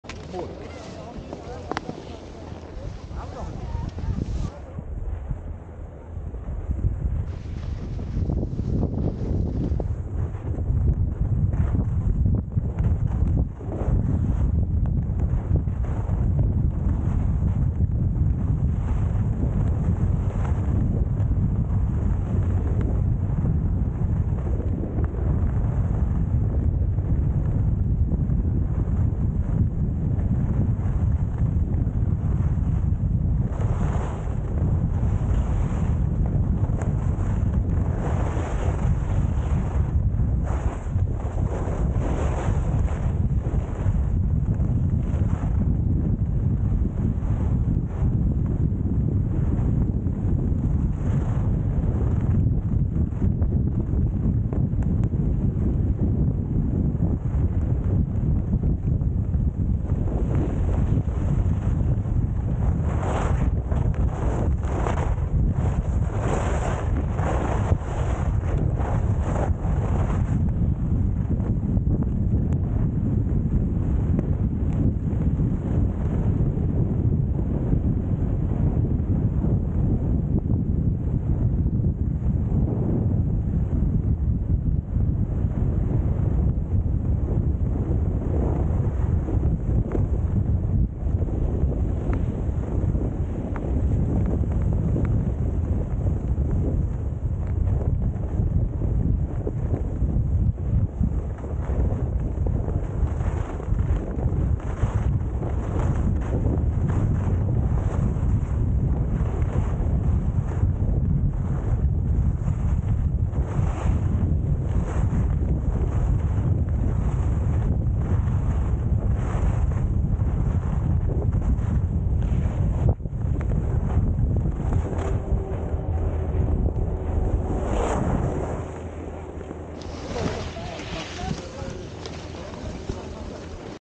Running the red Les Arcs Teppes piste.. it's red and fast :-).
Filmed with my Insta360 camera.